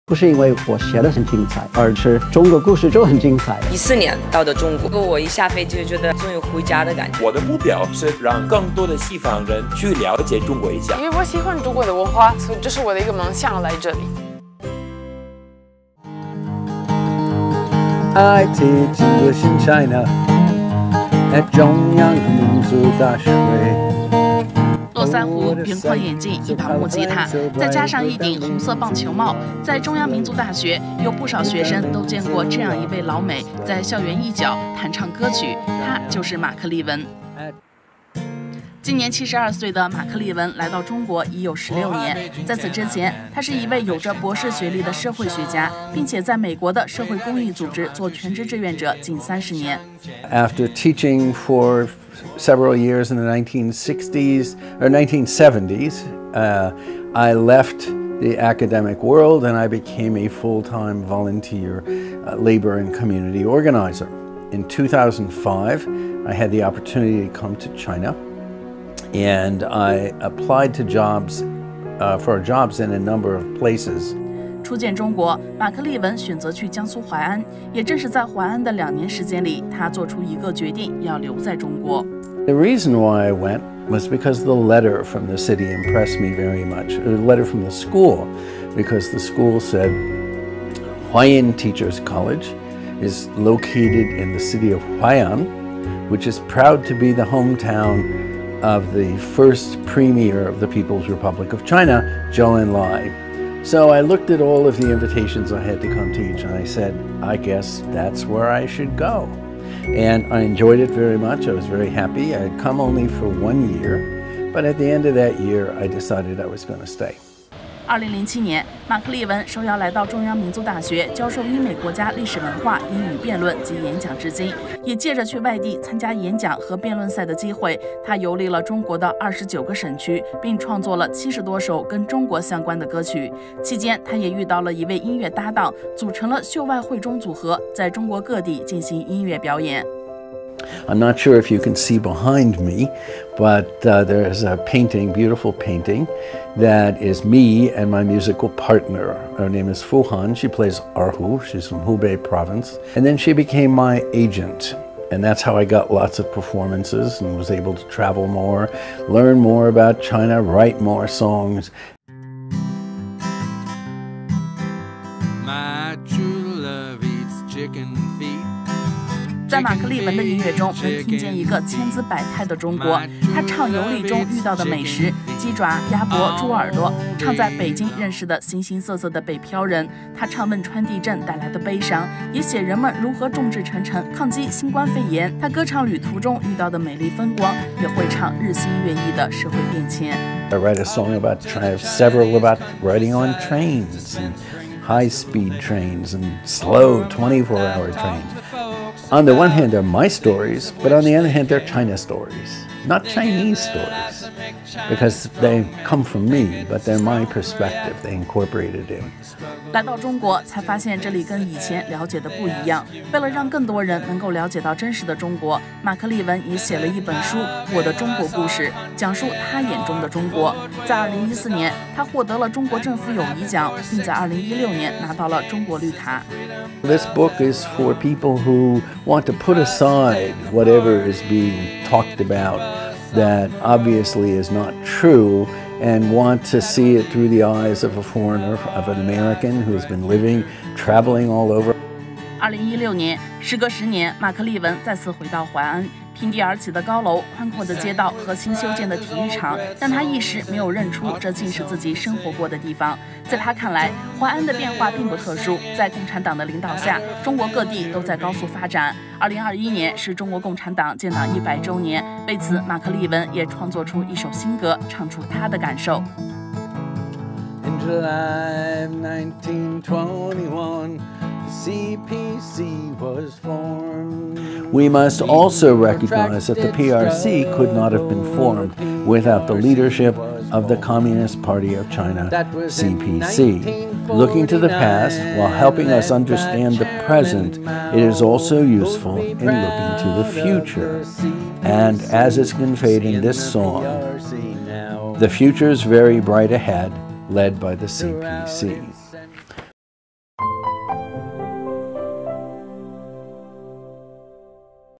拿中国“绿卡”的“老美”马克•力文： 用音乐展现真实中国
络腮胡、圆框眼镜，一把木吉他，再加上一顶红色棒球帽，在中央民族大学，有不少学生都见过这样一位“老美”在校园一角，弹唱歌曲。他，就是马克•力文。
今年72岁的马克•力文来到中国已经有16年，在此之前，他是一位有着博士学历的社会学家，并且在美国的社会公益组织做全职志愿者近三十年。